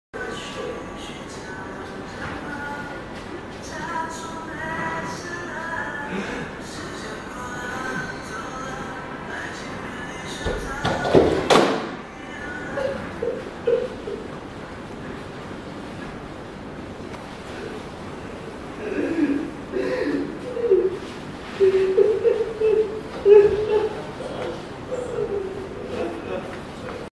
不可能重来·重来 DSCN2282.MOV
当人生，对活着感到没有意义时。。。他会有什么行动？
珍惜生命，预防自杀。